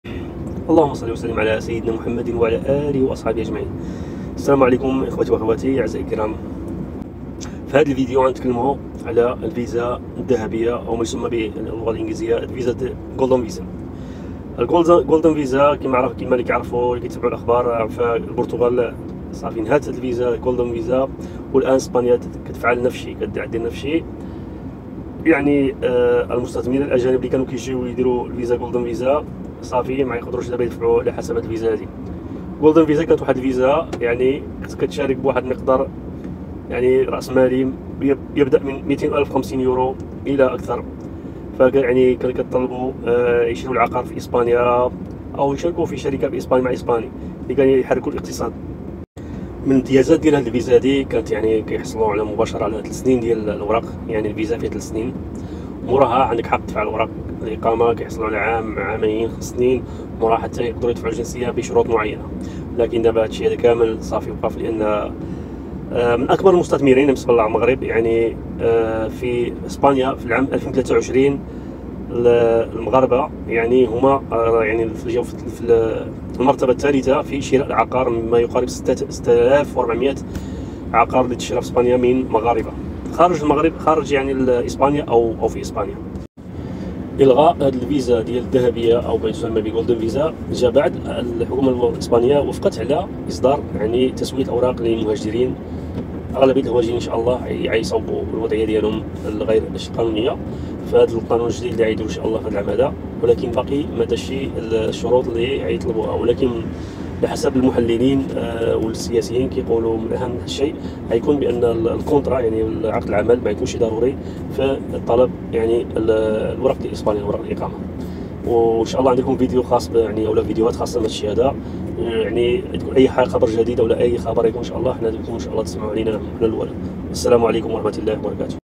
عبر فيزا الذهبية انتهت، لاكن سيتم تسوية أوضاع المهاجرين
السلام عليكم ورحمه الله وبركاته ، اللهم صلي وسلم وبارك على سيدنا محمد وعلى اله وصحبه اجمعين.